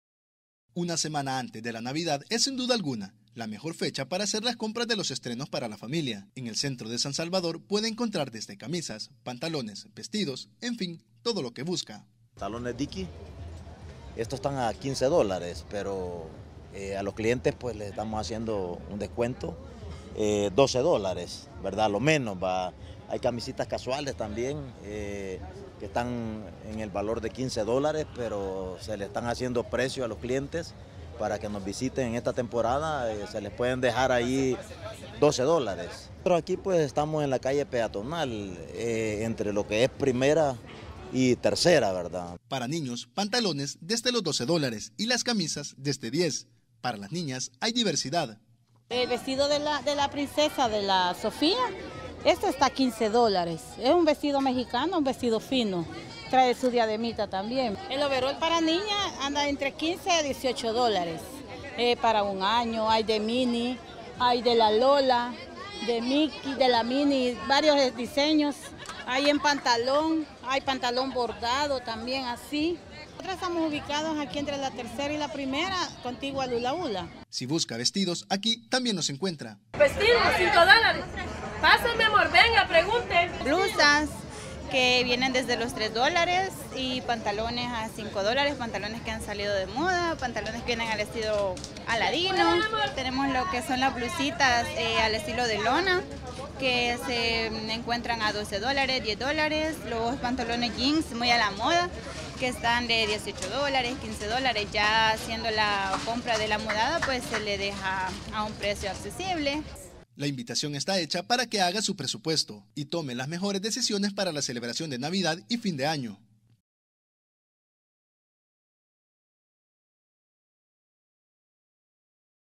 Estos son los precios para los tradicionales estrenos de fin de año
Estos son los precios para los tradicionales estrenos de fin de año en el Centro Histórico de San Salvador.